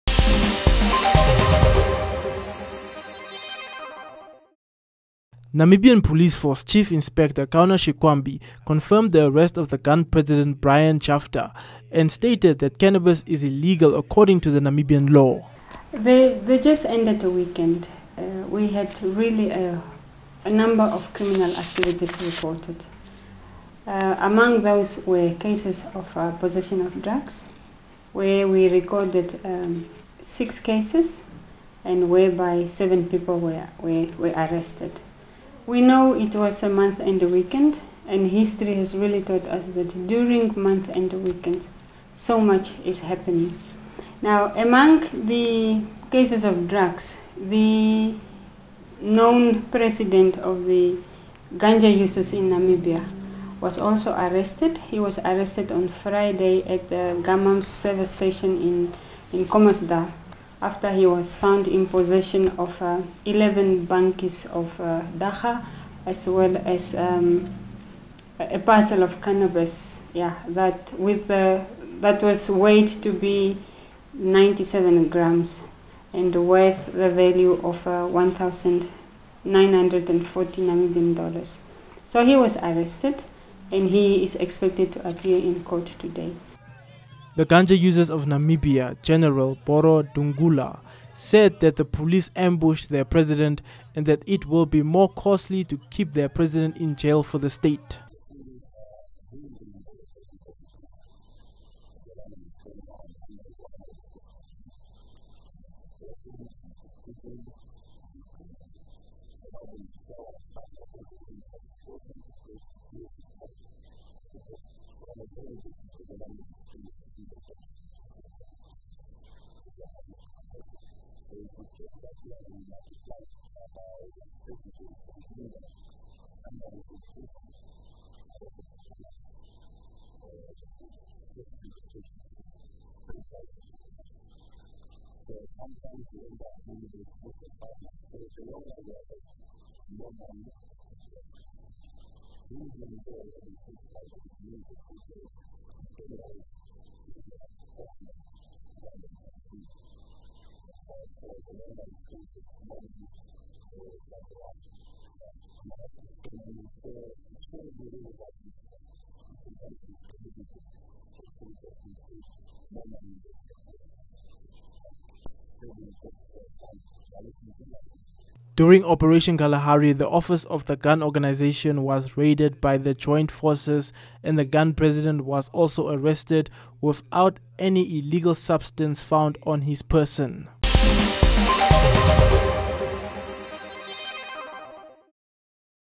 Police catches up with GUN president
The long arm of the law has caught up with the Ganja Users of Namibia(GUN) President, who was arrested by the Namibian Police on the 2 August 2019. The GUN President was in possession of 97grams of cannabis, to the value of N$ 1900.

Watch Today on One on DStv 284, GOtv Namibia 90 and NBC DTT 301 weekdays at 19h00.